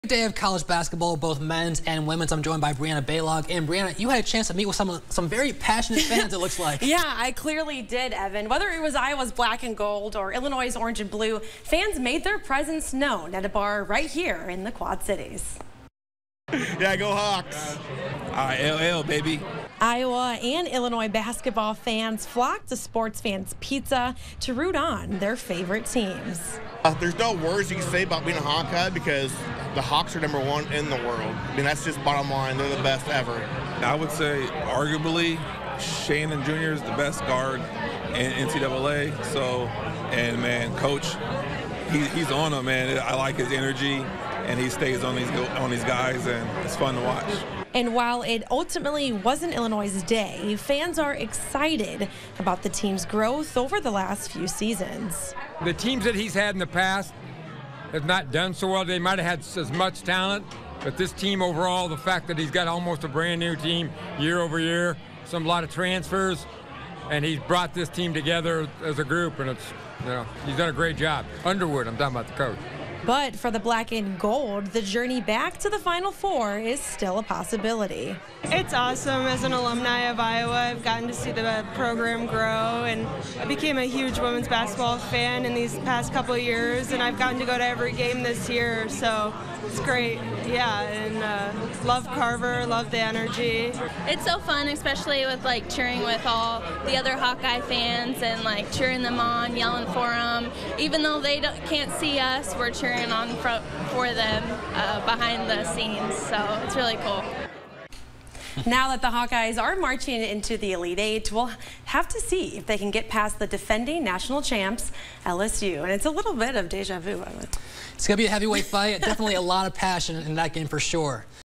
Iowa & Illinois fans take in March Madness
Iowa and Illinois basketball fans made their presence known at Sport’s Fans Pizza rooting on their favorite teams.